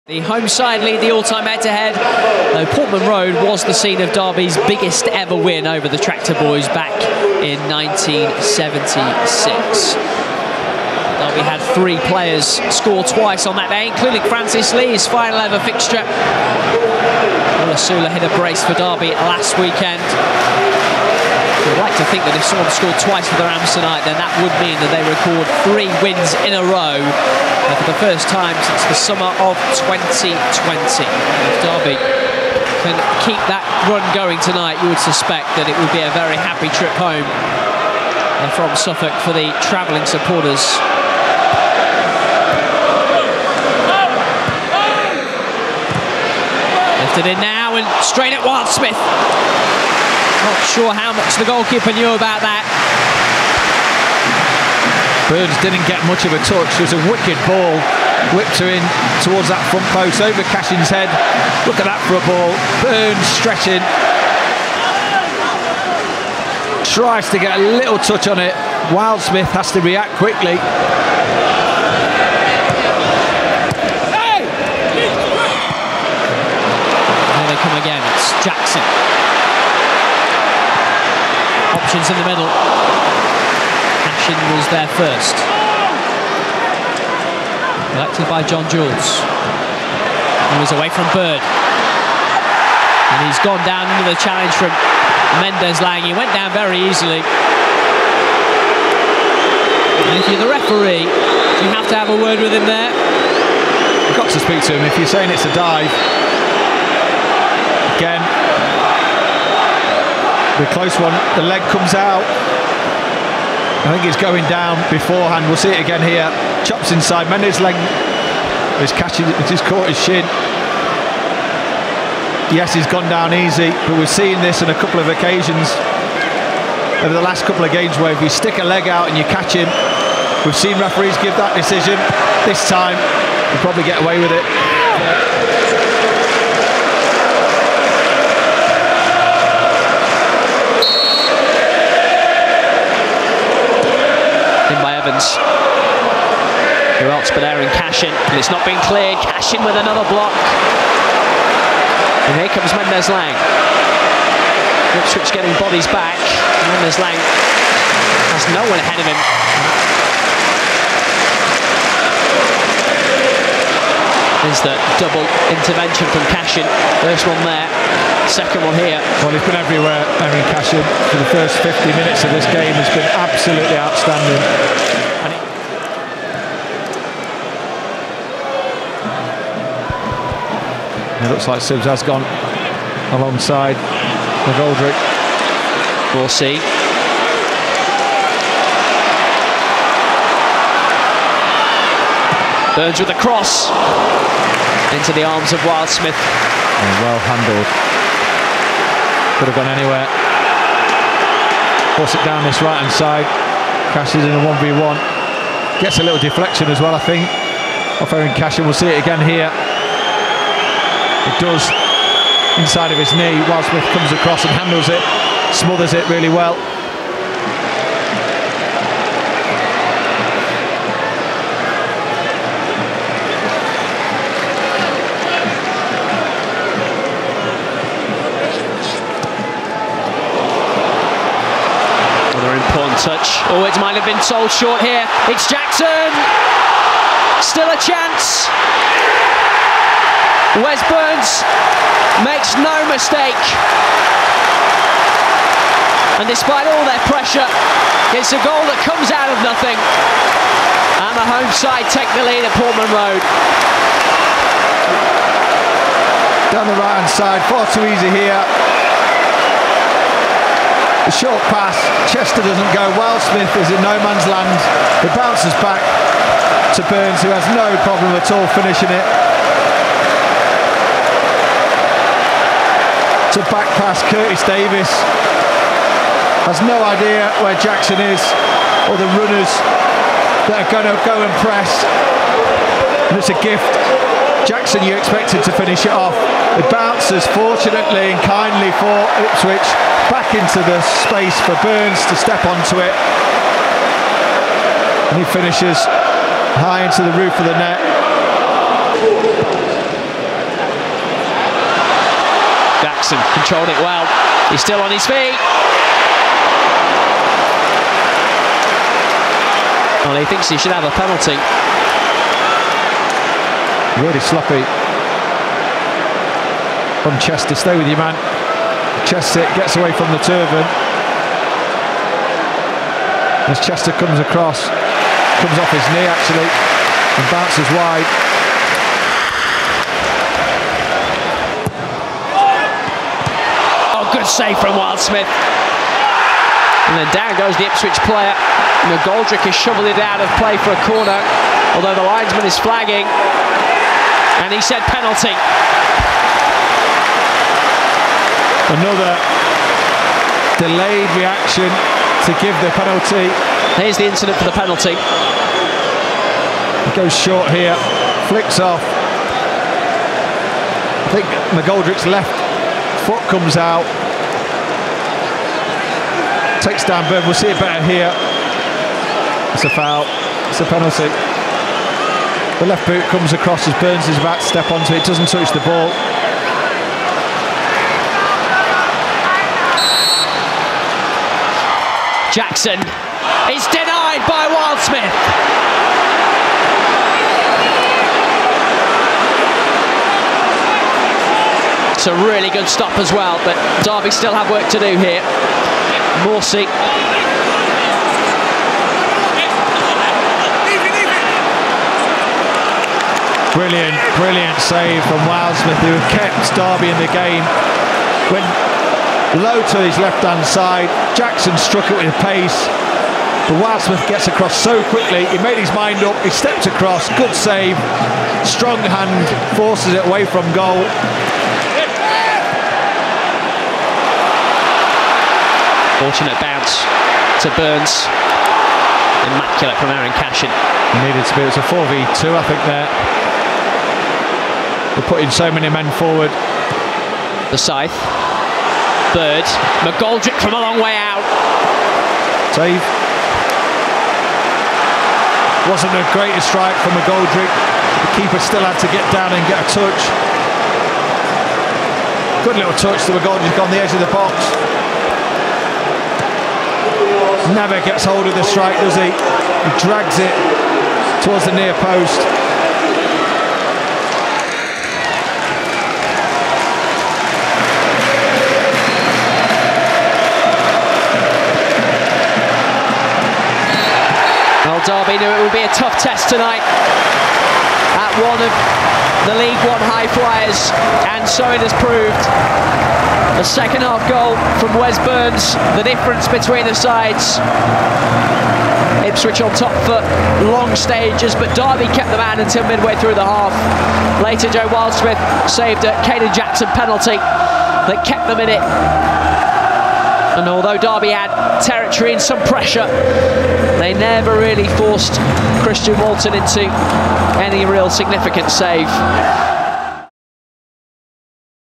HIGHLIGHTS I Ipswich Town Vs Derby County
Highlights from Derby County's Sky bet League One Away fixture to Ipswich Town.